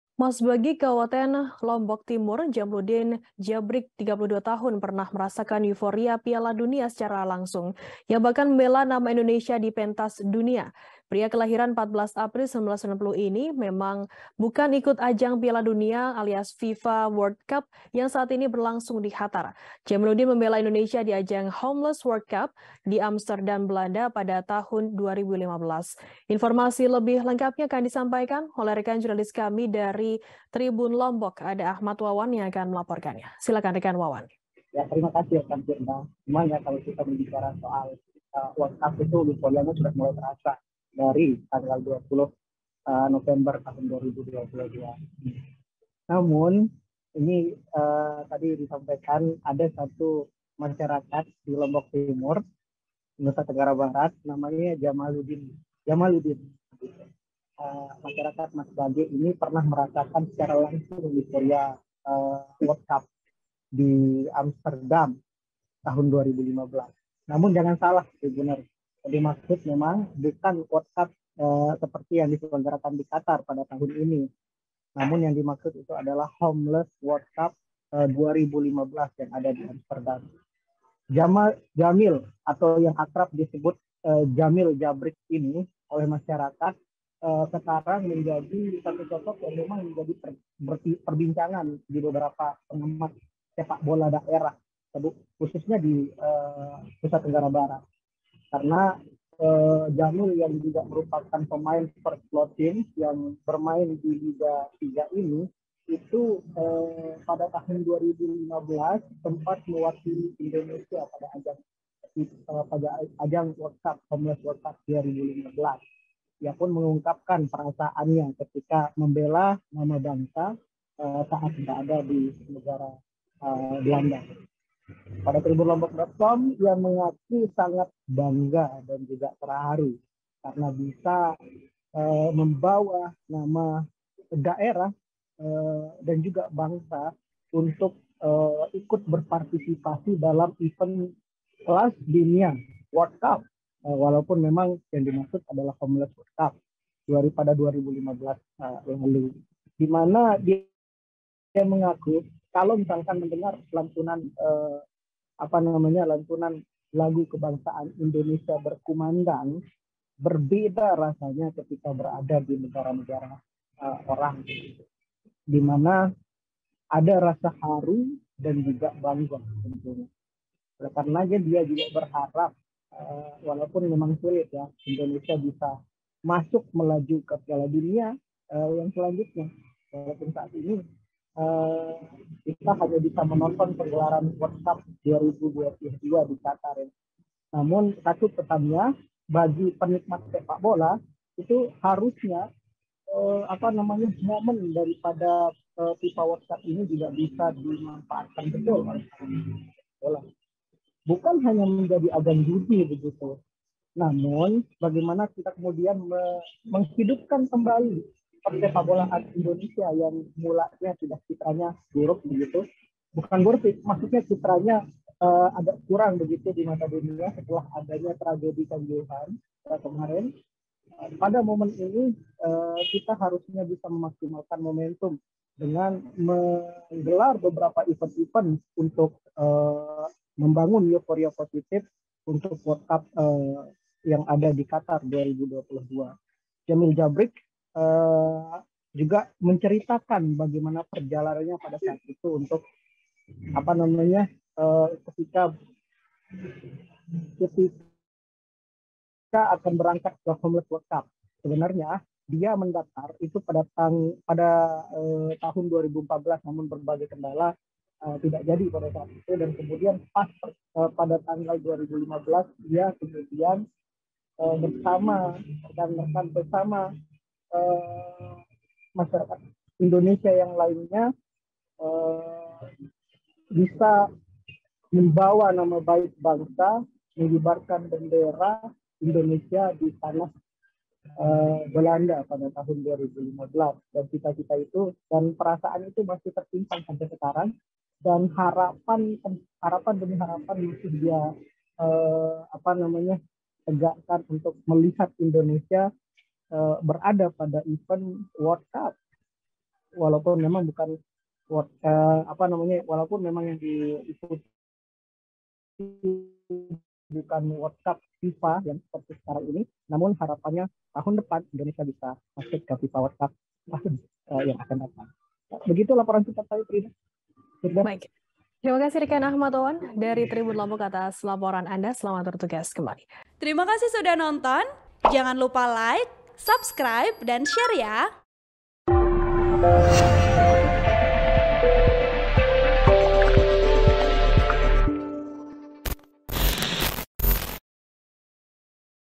Cerita Warga Lombok Timur yang Ikut Piala Dunia di Homeless World Cup Amsterdam Wakili Indonesia
TRIBUN-VIDEO.COM - Warga Masbagik, Kabupaten Lombok Timur, Jamludin Jabrik (32) pernah merasakan euforia Piala Dunia secara langsung. Dia bahkan membela nama Indonesia di pentas dunia.

Pria kelahiran 14 April 1990 ini memang bukan ikut ajang Piala Dunia alias FIFA World Cup yang saat ini berlangsung di Qatar.

Jamiludin membela Indonesia di ajang Homeless World Cup di Amsterdam, Belanda pada tahun 2015. (*)